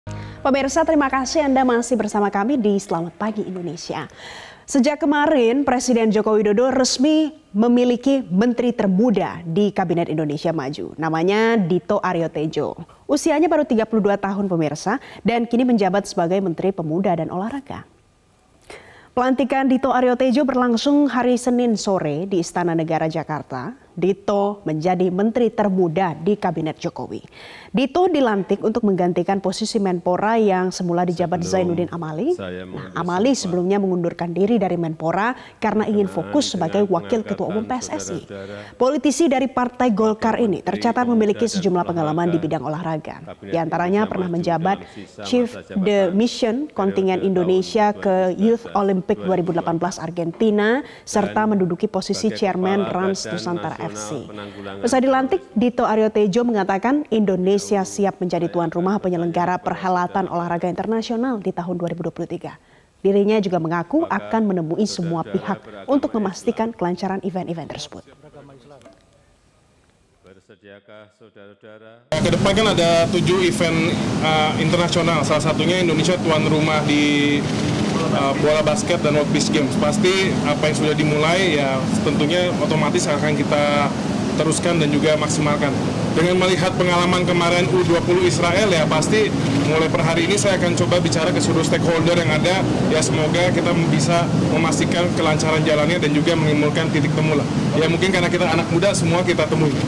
Dito Ariotedjo Resmi Dilantik Jadi Menpora
MetroTV, Sejak kemarin sore Presiden Joko Widodo resmi memiliki menteri termuda dikabinet Indonesia Maju yang bernama Dito Ariotedjo dan menjabat sebagai Menteri Pemuda dan Olahraga